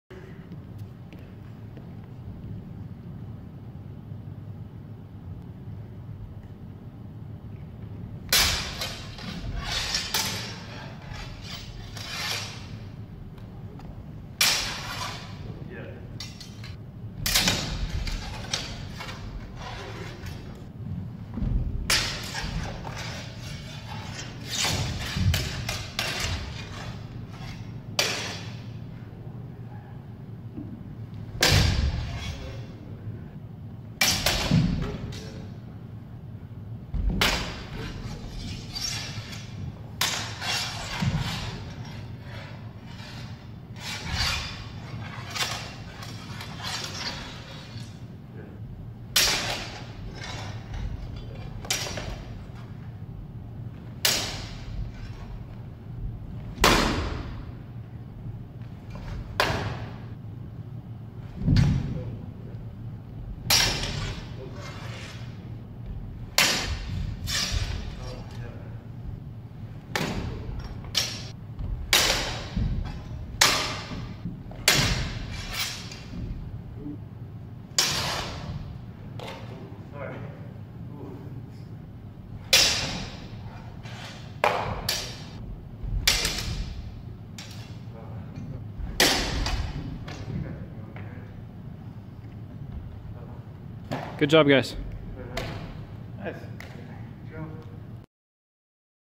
Ryan Harley 14 Aug 2019
Ryan and Harley doing some freeplay at the end of class